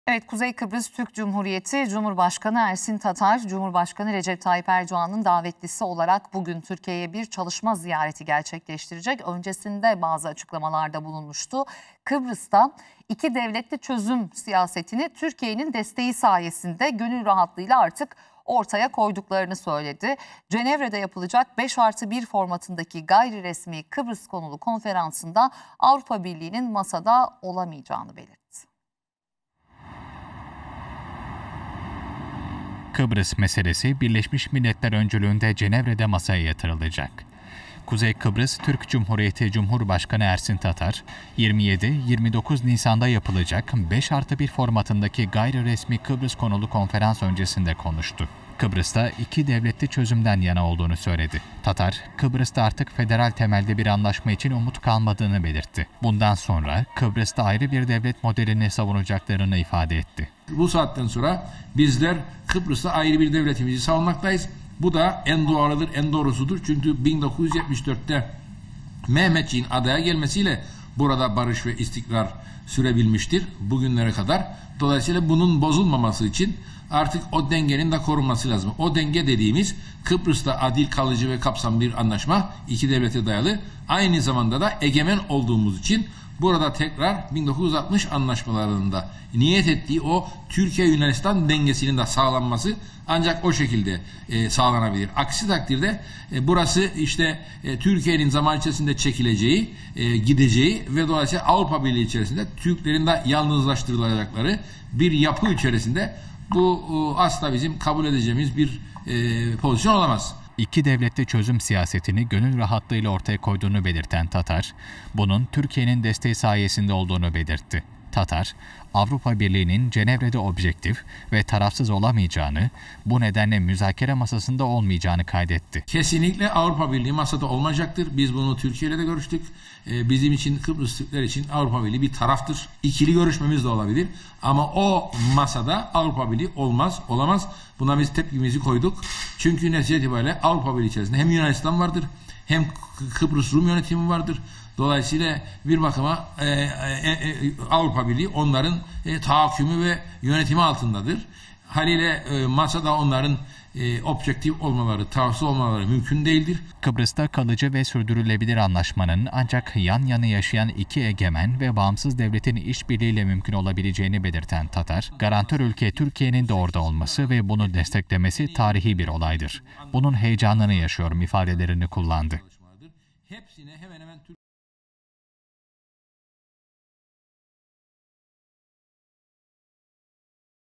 KKTC Cumhurbaşkanı Ersin Tatar'dan Türkiye'ye Teşekkür! Bugün Türkiye'ye Geliyor
KKTC Cumhurbaşkanı Ersin Tatar: İki devletli çözüm siyasetini Türkiye'nin desteğiyle yapıyorum.